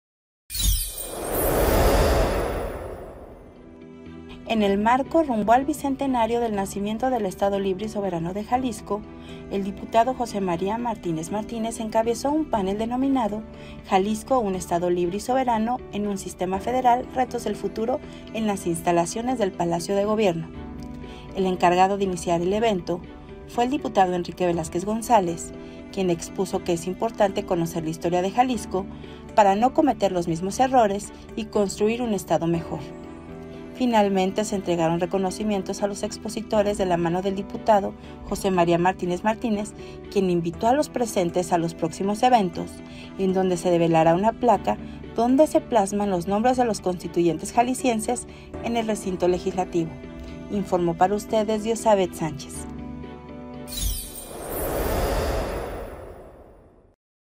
Congreso al Momento: Panel "Jalisco Estado Libre y Soberano."
En el marco Rumbo al Bicentenario del Nacimiento del Estado Libre y Soberano de Jalisco, el diputado José María Martínez Martínez encabezó un panel denominado: “Jalisco un estado libre y soberano en un sistema federal, retos del futuro” en las instalaciones del Palacio de Gobierno.

El encargado de iniciar el evento fue el diputado Enrique Velázquez González, quien expuso que es importante conocer la historia de Jalisco, para no cometer los mismos errores y construir un estado mejor.

Finalmente, se entregaron reconocimiento a los expositores de la mano del diputado José Maris Martínez Martínez quien invitó a los presentes a los próximos eventos en donde se debelará una placa donde se plasman los nombres de los Constituyentes Jaliscienses en el Recinto Legislativo.